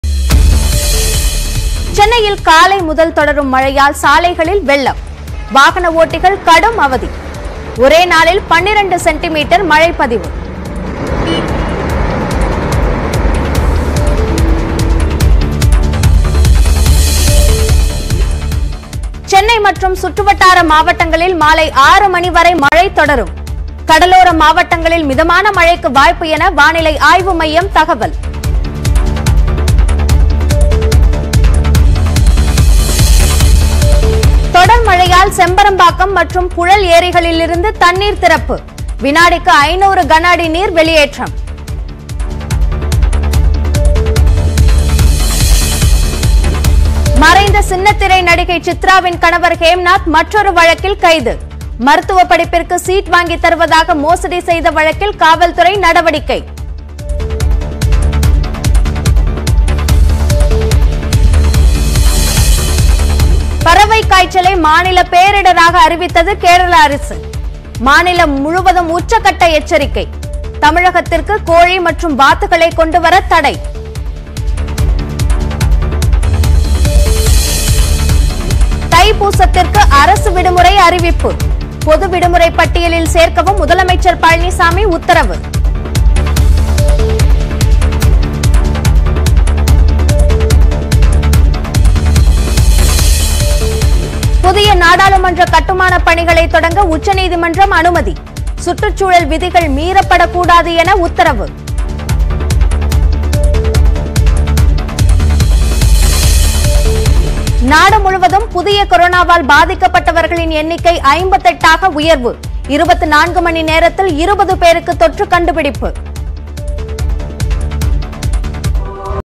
தற்போதைய நேரத்தின் தலைப்புச் செய்திகள் | Today Headlines | News18 Tamil Nadu Headlines | 05.01.2021
0:03 - ஒரே நாளில் 12 செ.மீ., மழை
0:19 - சென்னையில் மழை தொடரும்
0:35 - செம்பரம்பாக்கம், புழல் ஏரிகள் திறப்பு
0:47 - மோசடி வழக்கில் ஹேம்நாத் கைது
1:03 - பறவைக்காய்ச்சல் பேரிடர் - மாநில அரசு
1:17 - தைப்பூசத்திற்கு அரசு விடுமுறை
1:34 - புதிய நாடாளுமன்றம் கட்ட அனுமதி